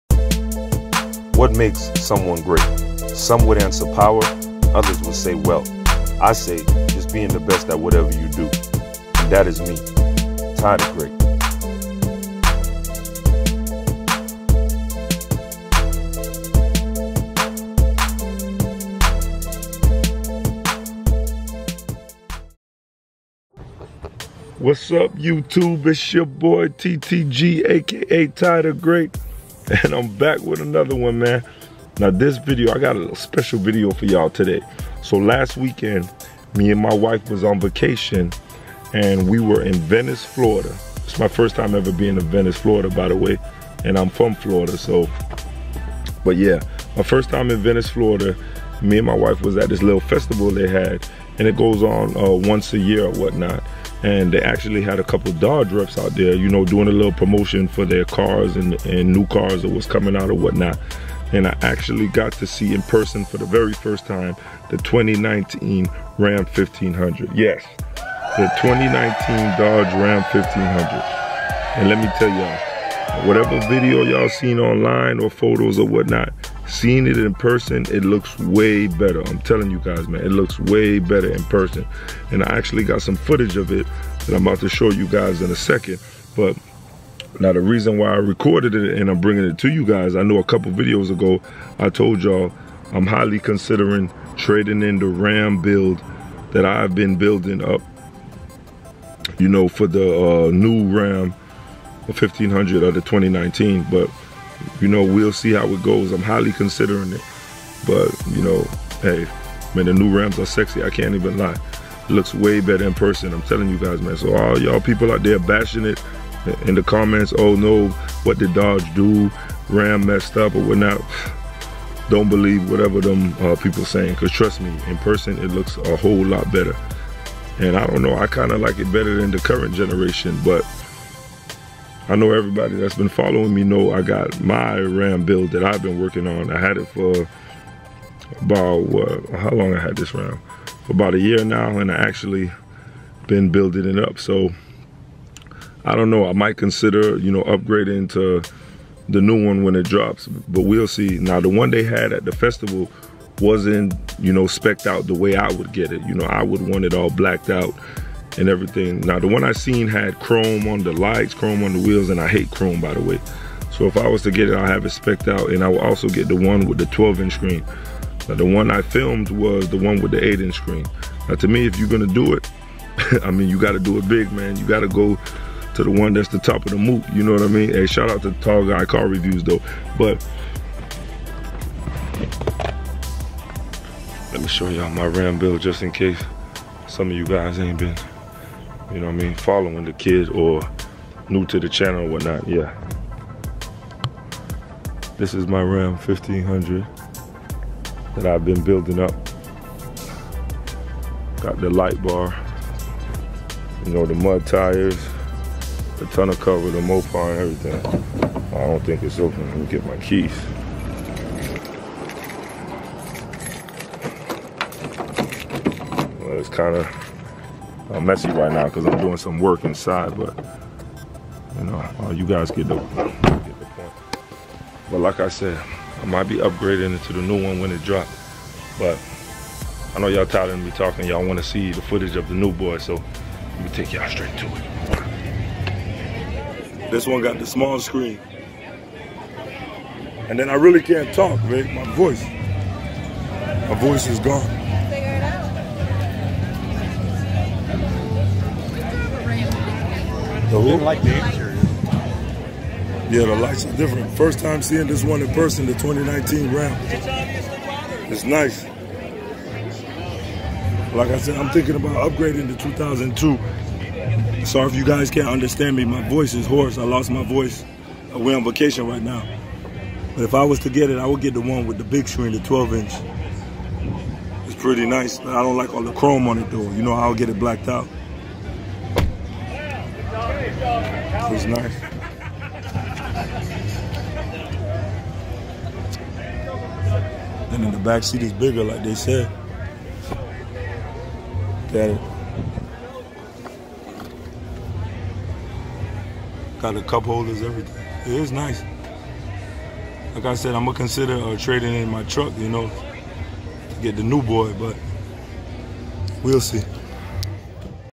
Seen The New 2019 Dodge Ram 1500 In Person!!! Im Buying it!!!!
Ran into the new redesigned 2019 Dodge Ram 1500.Will I upgrade??